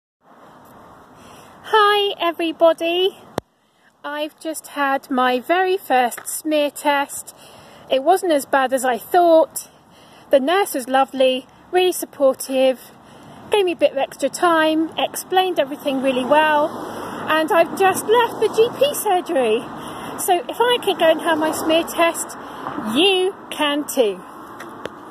My experience of going for a Smear Test
Me having for a smear test